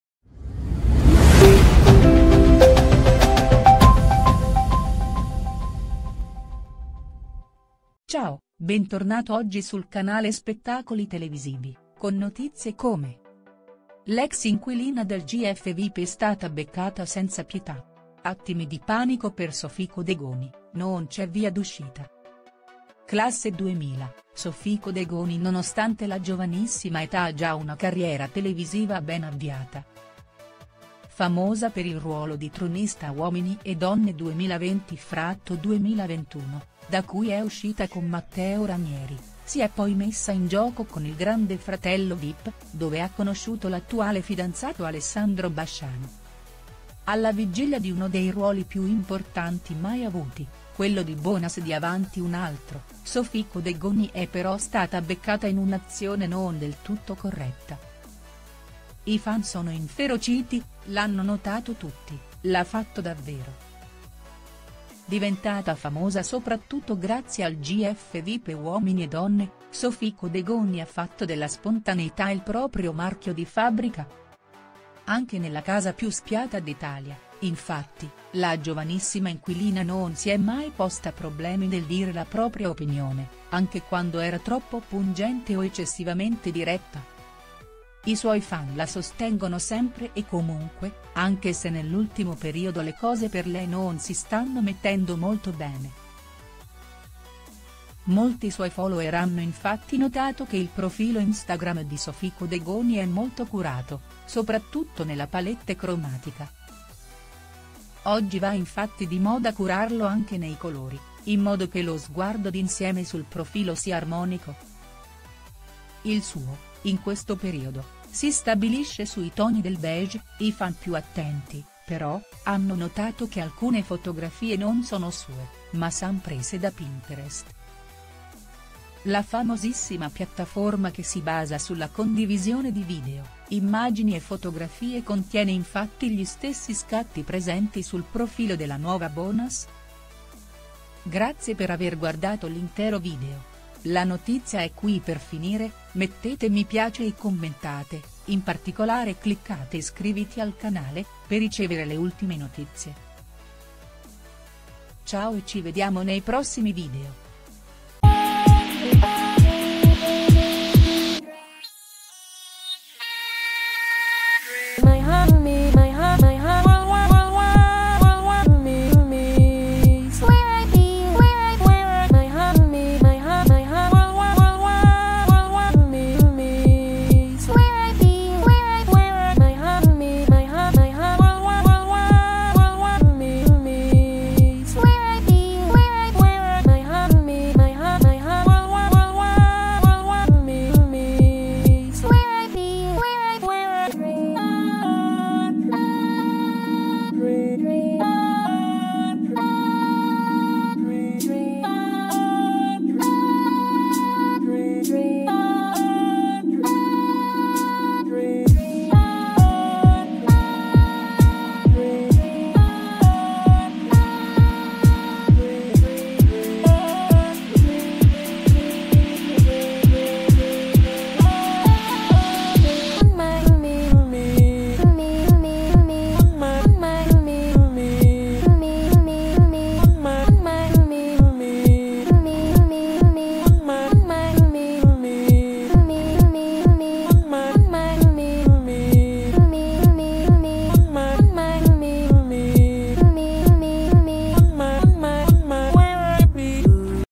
Sophie Codegoni colta in flagrante “Voto zero meno!” si becca un’accusa gravissima
Classe 2000, Sophie Codegoni nonostante la giovanissima età ha già una carriera televisiva ben avviata. Famosa per il ruolo di tronista a Uomini e Donne 2020/2021, da cui è uscita con Matteo Ranieri, si è poi messa in gioco con il Grande Fratello VIP, dove ha conosciuto l’attuale fidanzato Alessandro Basciano.

Alla vigilia di uno dei ruoli più importanti mai avuti, quello di Bonas di Avanti Un Altro, Sophie Codegoni è però stata beccata in un’azione non del tutto corretta. I fan sono inferociti, l’hanno notato tutti: l’ha fatto davvero.